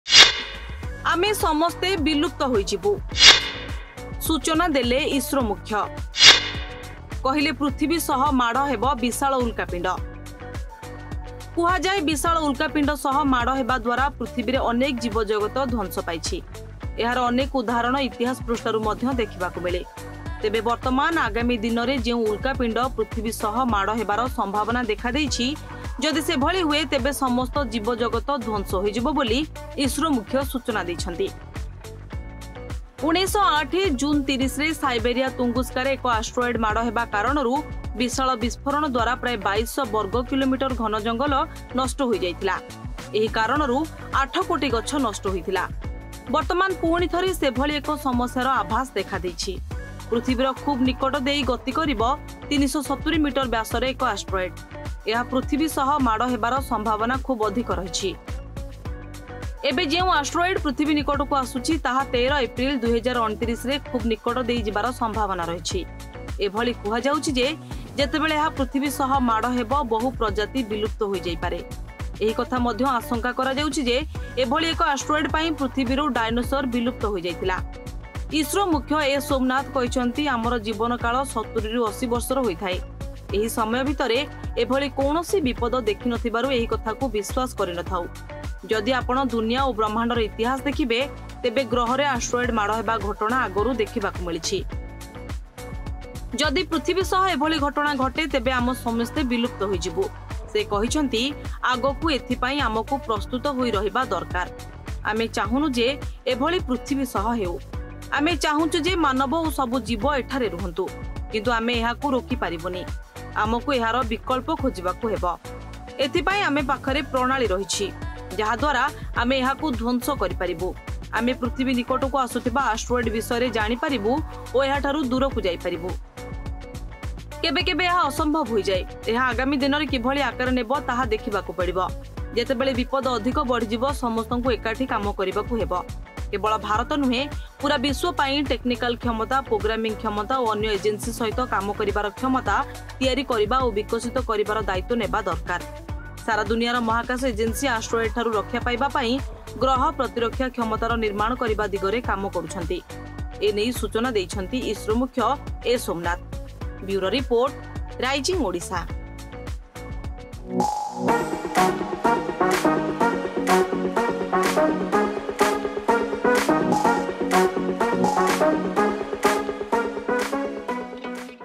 ISRO Chief S Somanath Warns 'Asteroid Hitting Earth A Real Possibility...' | Details About Asteroids
Watch Details About Asteroids Impact On Earth.